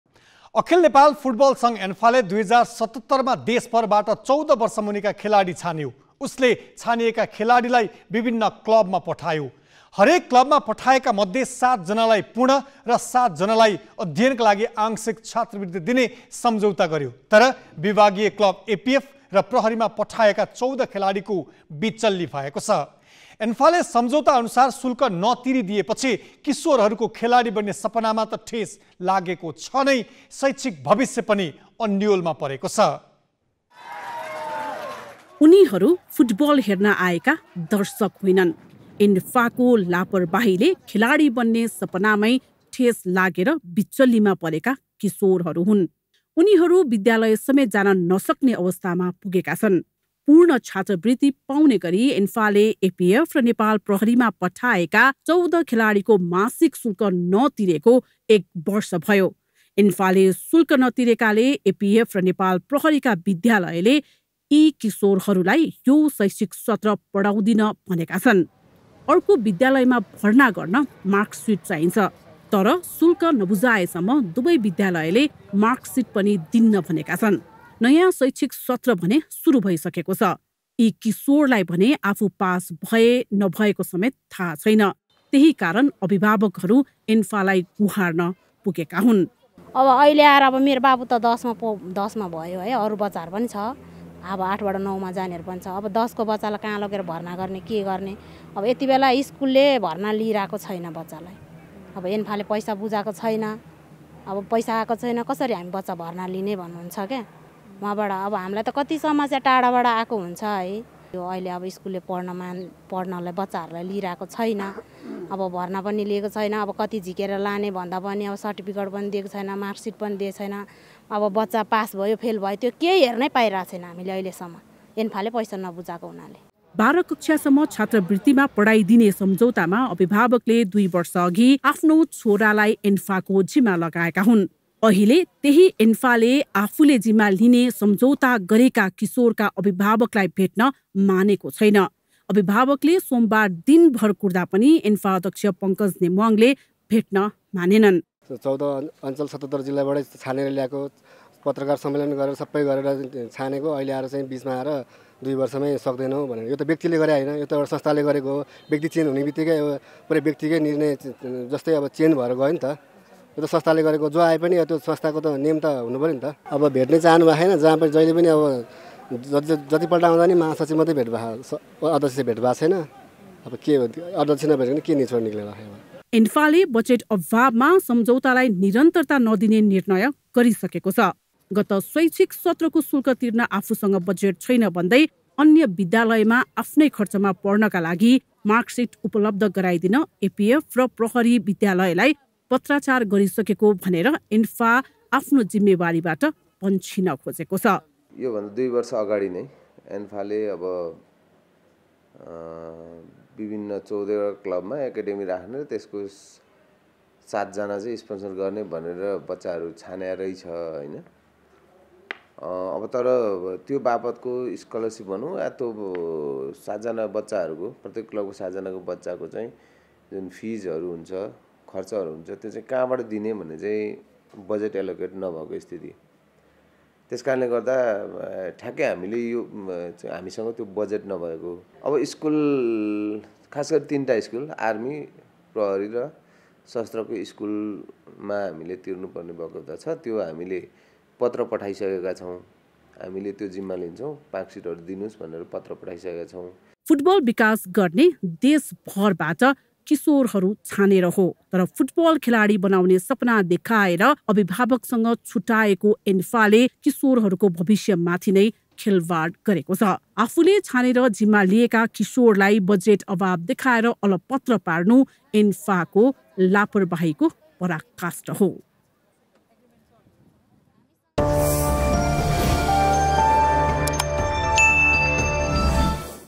एन्फाले सम्झौता अनुसार शुल्क नतिरेपछि किशोरहरुको खेलाडी बन्ने सपनामा ठेस, शैक्षिक भविष्य अन्योलमा
एन्फाले सम्झौता अनुसार शुल्क नतिरेपछि किशोरहरुको खेलाडी बन्ने सपनामा ठेस, शैक्षिक भविष्य पनि अन्योलमा | Galaxy News Report

अखिल नेपाल फुटबल संघ एन्फाले २०७७ मा देशभरबाट १४ वर्षमुनिका खेलाडी छान्यो । उसले छानिएका खेलाडीलाई विभिन्न क्लबमा पठायो । हरेक क्लबमा पठाइएकामध्ये ७ जनालाई पूर्ण र ७ जनालाई अध्ययनका लागि आंशिक छात्रवृत्ति दिने सम्झौता गर्‍यो । तर विभागीय क्लब एपीएफ र प्रहरीमा पठाइएका १४ खेलाडीको विचल्ली भएको छ । एन्फाले सम्झौता अनुसार शुल्क नतिरेपछि किशोरहरुको खेलाडी बन्ने सपनामा त ठेस लागेको छ नै, शैक्षिक भविष्य पनि अन्योलमा परेको छ ।